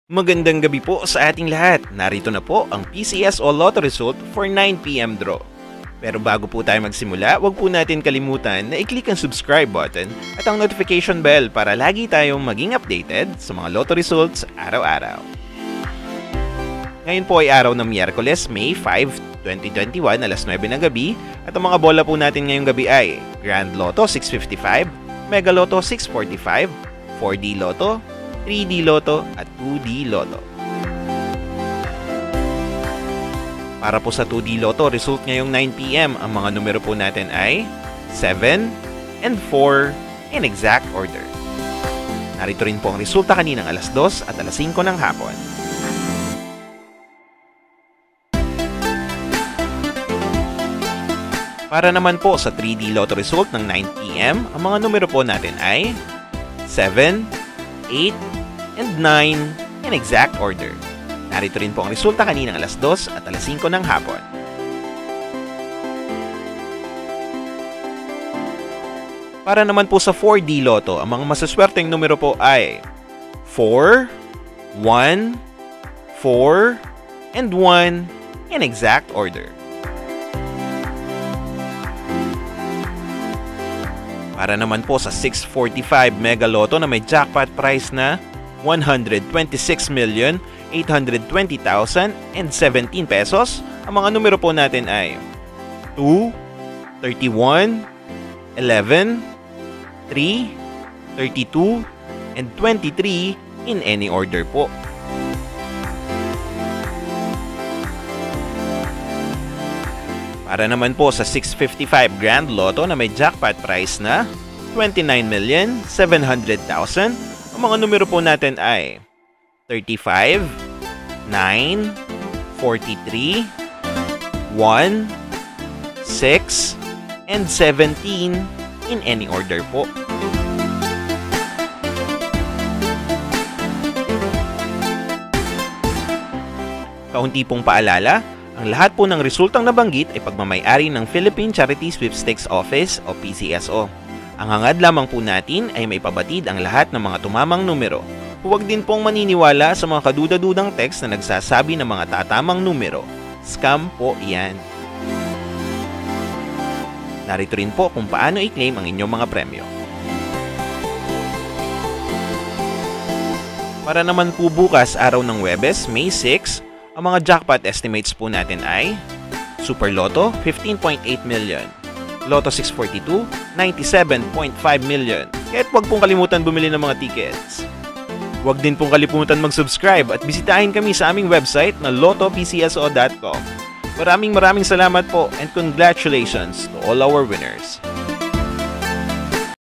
Lotto Result Today 9pm Draw May 5, 2021 PCSO 6/55 6/45 4D Swertres/3D EZ2/2D
Here are the PCSO Lotto Results for May 5, 2021 for 6/55 6/45 4D Swertres and EZ2 Lotto.